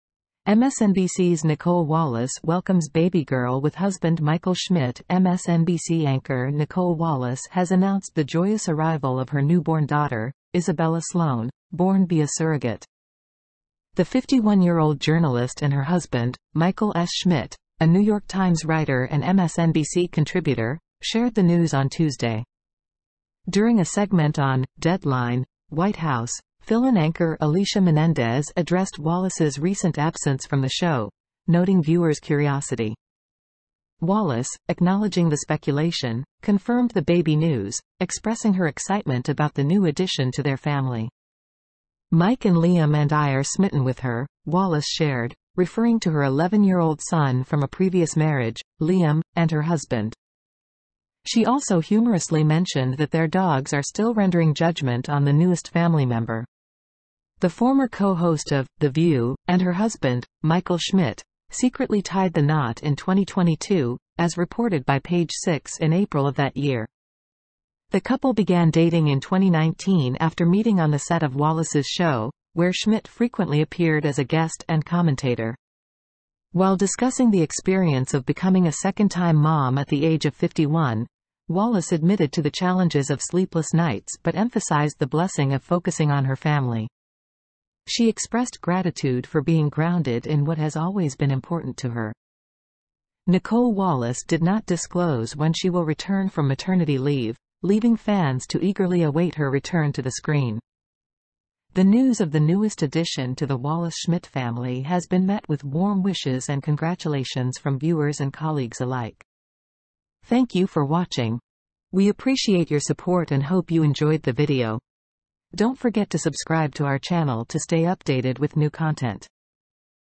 Family Bliss: Nicolle Wallace and Michael Schmidt Embrace Parenthood Again
Nicolle Wallace and Michael Schmidt, hosts of MSNBC, Have a Baby Girl.
Nicolle Wallace, an MSNBC journalist, is celebrating the birth of her surrogate baby, Isabella Sloane. Michael S. Schmidt, a writer for the New York Times and a contributor to MSNBC, and his wife, the 51-year-old journalist, broke the news on Tuesday.
Alicia Menendez, who was filling in for host Chris Wallace on "Deadline: White House," acknowledged the wonderment of viewers when she discussed Wallace's recent absence. In response to the rumors, Wallace confirmed the pregnancy and shared her joy at welcoming a new member to their family.
"Mike and Liam and I are smitten with her," Wallace revealed, mentioning her husband Mike and her 11-year-old son Liam from a prior marriage. Additionally, she made the amusing observation that their canines are still passing judgment on the new addition to the family.
According to an April 2022 Page Six report, the former "The View" co-host and her spouse Michael Schmidt discreetly married the knot. Schmidt was a regular guest and pundit on Wallace's show, and the two started dating in 2019 after meeting on set.
Wallace acknowledged the difficulties of sleepless nights but stressed the blessing of focusing on her family while addressing her experience of being a second-time mom at the age of 51. Being rooted in what has always mattered to her was something she was grateful for, she said.
Nicolle Wallace has kept her return from maternity leave a secret, so her admirers are waiting impatiently for her to grace their screens again. Viewers and coworkers alike have expressed their joy and well wishes upon hearing of the Wallace-Schmidt family's latest arrival.